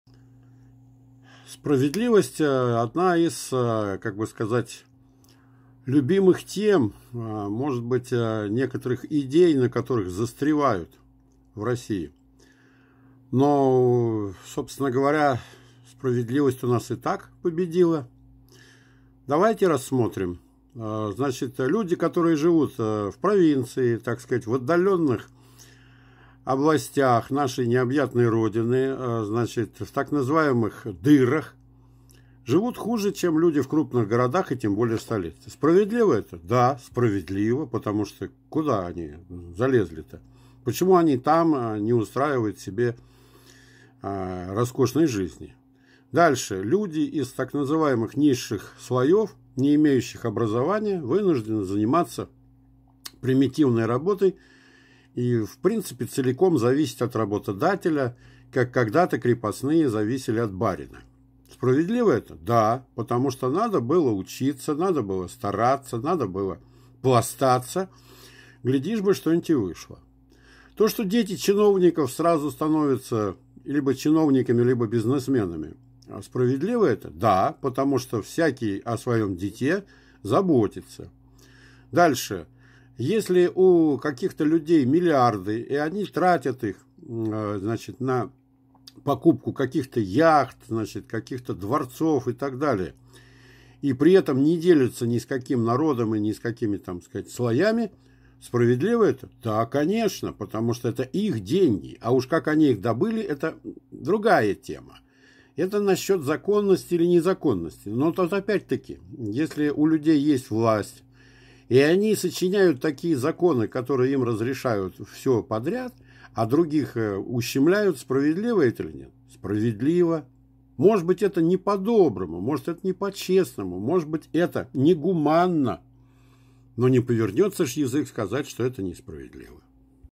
СПРАВЕДЛИВОСТЬ в РОССИИ... а оно надо?
И так всё справедилово: у кого власть + деньги и наоборот, тем хорошо